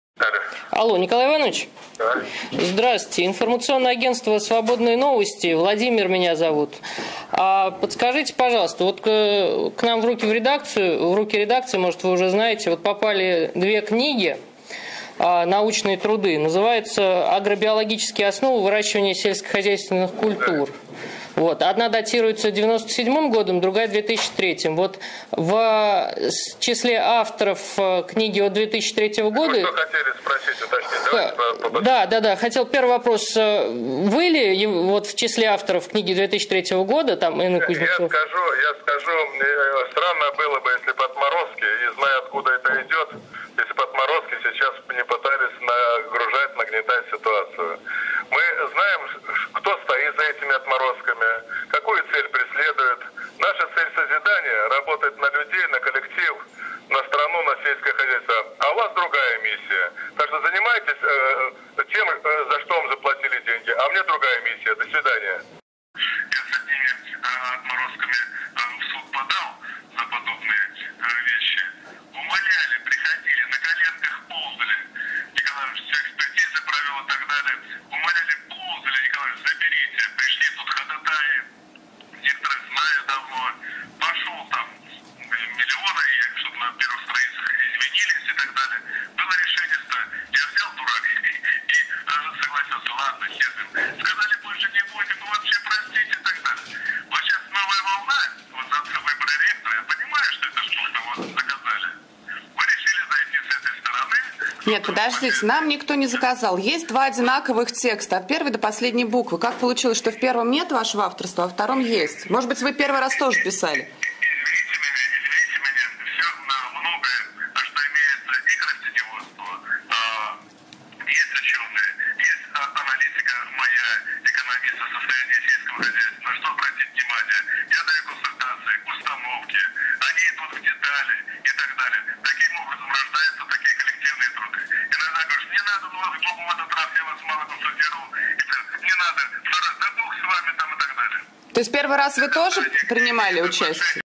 Ректор СГАУ Николай Кузнецов по телефону
Ректор СГАУ Николай Кузнецов общается по телефону с журналистами ИА "Свободные новости".